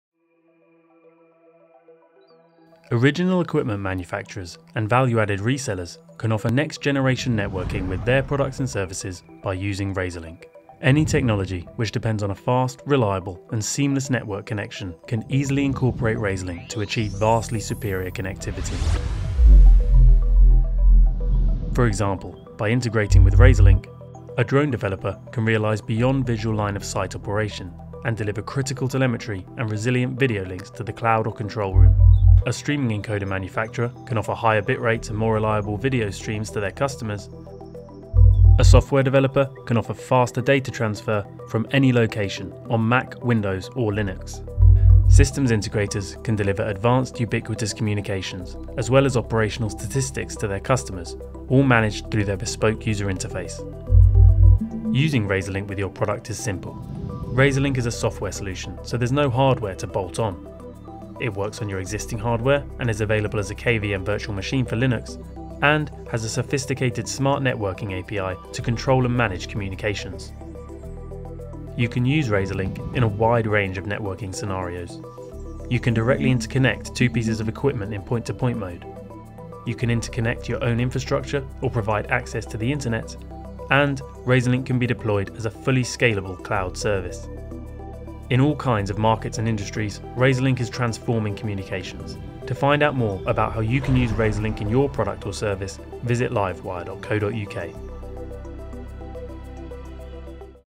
RazorLink for OEM's & VAR's
Original Equipment Manufacturers and Value Added Resellers can offer next generation networking with their products and services by using RazorLink.

Any technology which depends on a fast, reliable and seamless network connection can easily incorporate RazorLink to achieve vastly superior connectivity

For example, by integrating with RazorLink…

A drone developer can realise Beyond Visual Line Of Sight operation, and deliver critical telemetry and resilient video links to the cloud or control room

A streaming encoder manufacturer can offer higher bit rates and more reliable video streams to their customers

A software developer can offer faster data transfer, from any location, on mac, windows or linux

Systems Integrators can deliver advanced ubiquitous communications, as well as operational statistics to their customers, all managed through their bespoke user interface

Using RazorLink with your product is simple…

• RazorLink is a software solution, so there’s no hardware to “bolt on"
• It works on your existing hardware and is available as a KVM Virtual machine for Linux
• And  has a sophisticated Smart Networking API to control and manage Communicatons

You can use RazorLink in a wide range of networking scenarios…
• You can directly interconnect two pieces of equipment in “point to point” mode
• You can interconnect your own infrastructure or provide access to the Internet
• And RazorLink can be deployed as a fully scalable cloud service

In all kinds of markets and industries RazorLink is transforming communications, to find out more about how you can use RazorLink in your product or service visit Livewire.co.uk